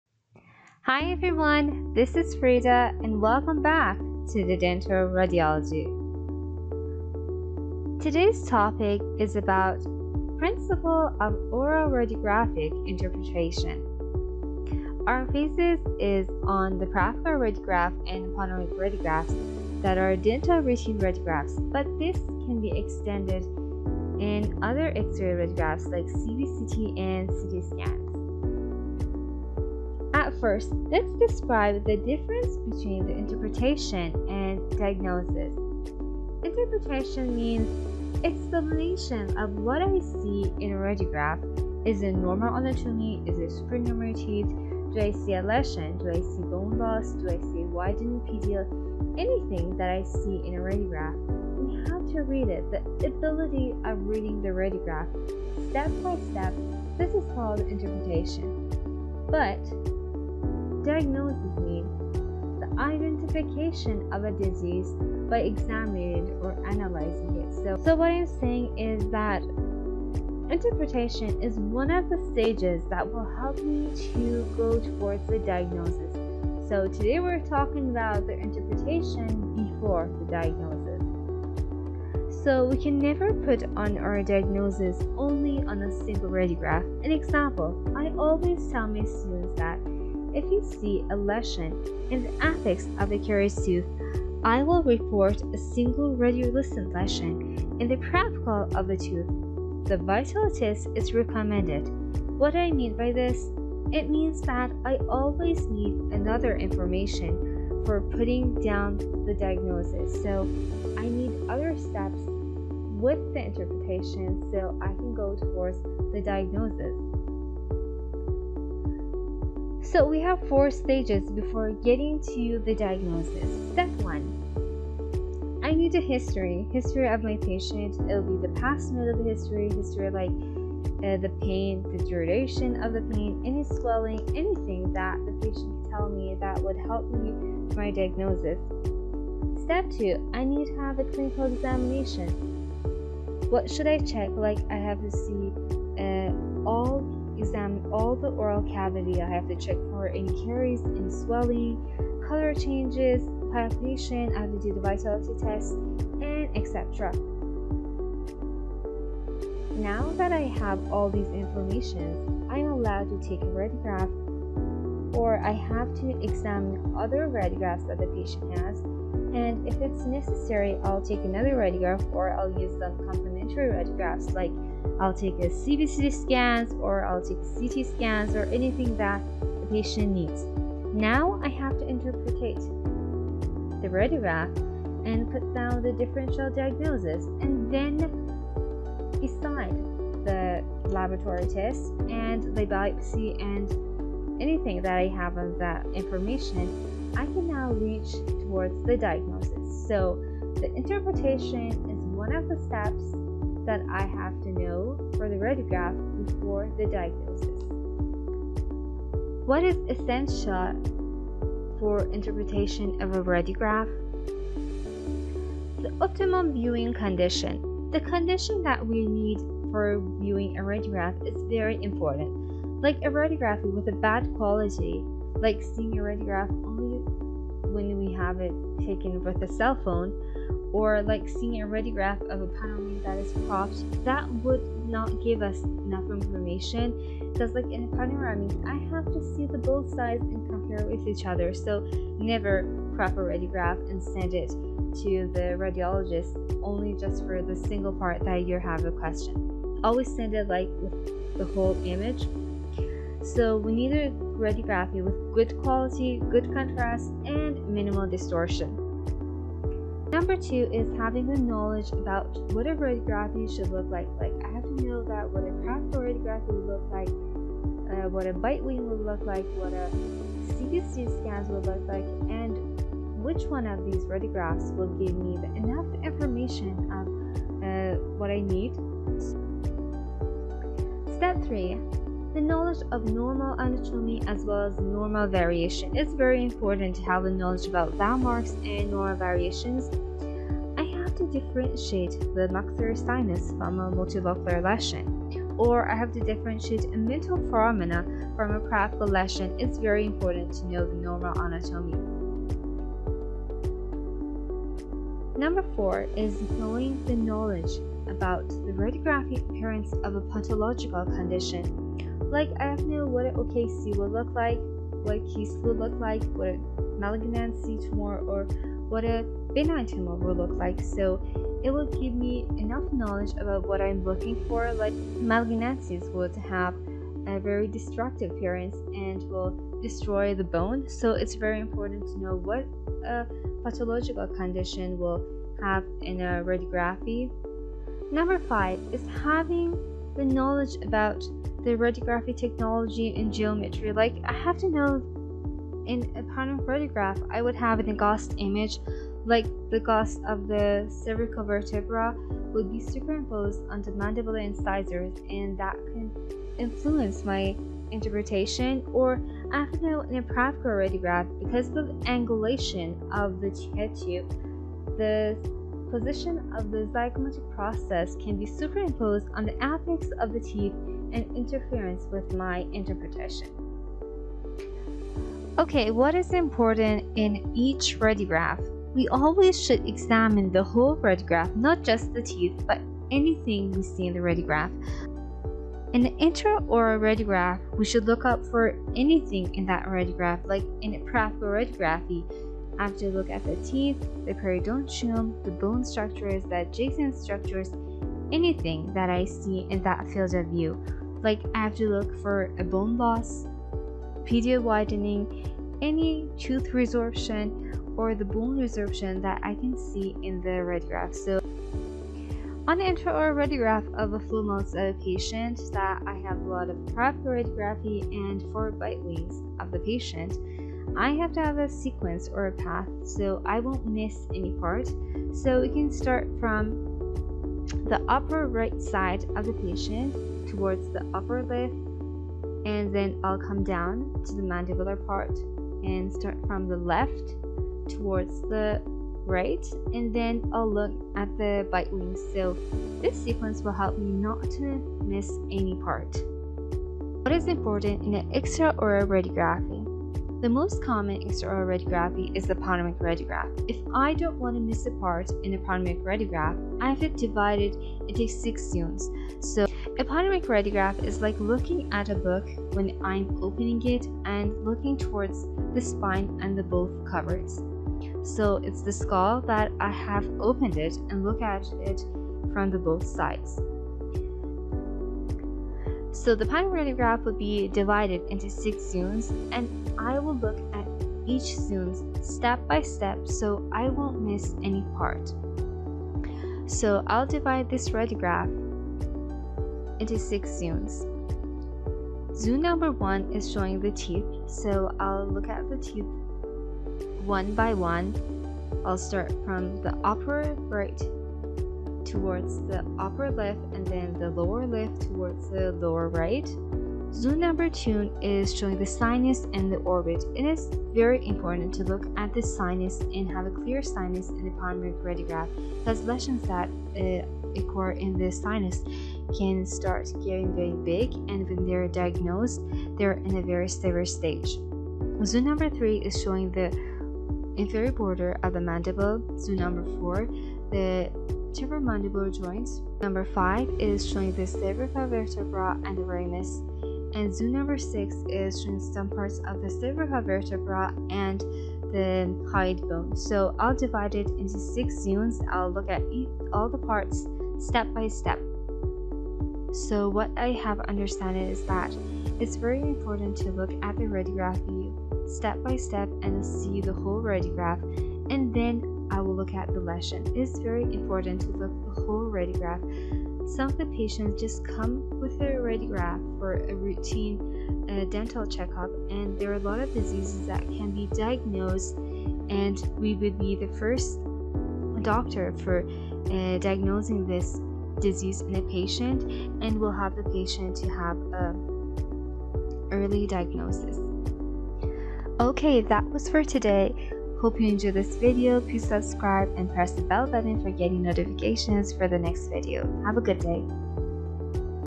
Principles of Radiographic Interpretation | Oral Radiology
This video is about radiographic interpretation.
How to start interpreting a dental radiograph.
The Principles of Radiographic Interpretation is a very important step before diagnosis.